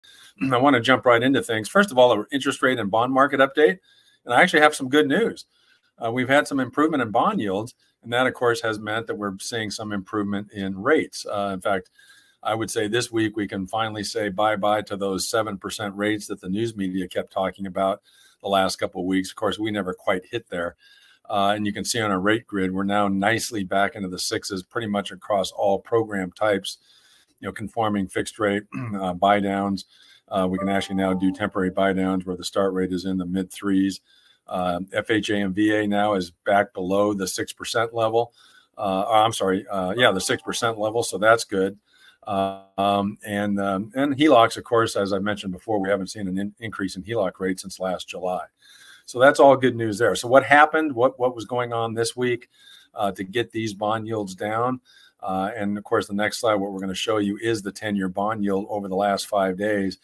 Interest Rate and Bond Market Update
Exciting news! Bond yields have improved, bringing interest rates down. We're back in the 6% range, with FHA and VA loans below 6%. Watch our latest update for more info!